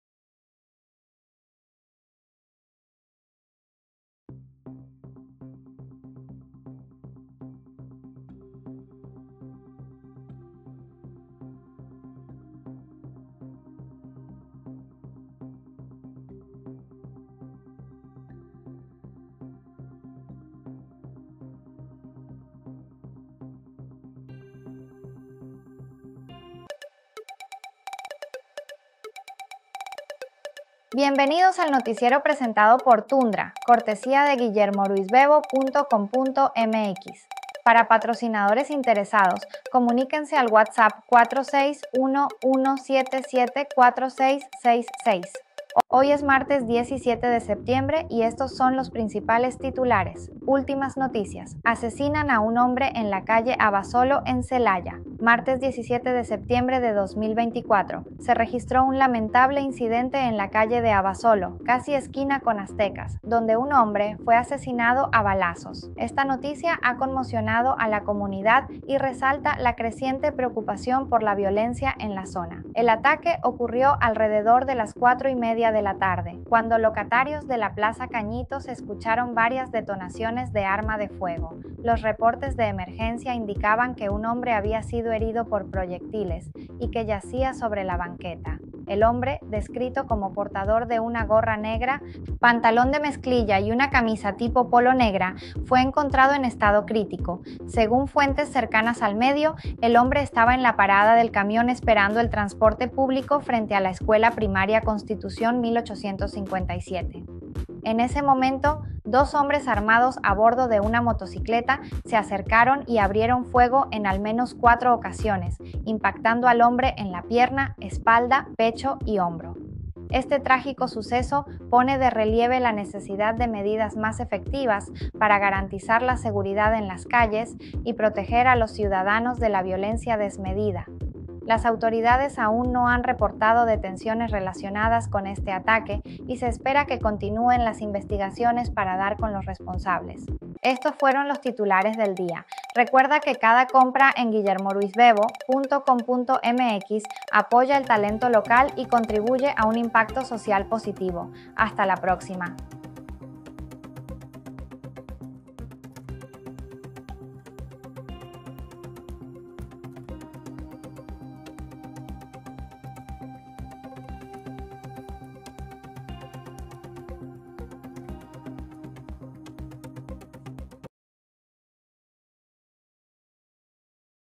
Noticiero Con Tundra Martes 17 De Septiembre 2024.
Últimas Noticias: Asesinan a un Hombre en la calle Abasolo de Celaya.

En el día de ayer, martes 17 de septiembre de 2024, se registró un lamentable incidente en la calle de Abasolo, casi esquina con Aztecas, donde un hombre fue asesinado a balazos. Esta noticia ha conmocionado a la comunidad y resalta la creciente preocupación por la violencia en la zona.
El ataque ocurrió alrededor de las cuatro y media de la tarde, cuando locatarios de la Plaza Cañitos escucharon varias detonaciones de arma de fuego. Los reportes de emergencia indicaban que un hombre había sido herido por proyectiles, y que yacía sobre la banqueta. El hombre, descrito como portador de una gorra negra, pantalón de mezclilla y una camisa tipo polo negra, fue encontrado en estado crítico.

Según fuentes cercanas al medio, el hombre estaba en la parada del camión esperando el transporte público frente a la escuela primaria Constitución 1857. En ese momento, dos hombres armados a bordo de una motocicleta se acercaron y abrieron fuego en al menos cuatro ocasiones, impactando al hombre en la pierna, espalda, pecho y hombro.

Este trágico suceso pone de relieve la necesidad de medidas más efectivas para garantizar la seguridad en las calles y proteger a los ciudadanos de la violencia desmedida. Las autoridades aún no han reportado detenciones relacionadas con este ataque, y se espera que continúen las investigaciones para dar con los responsables.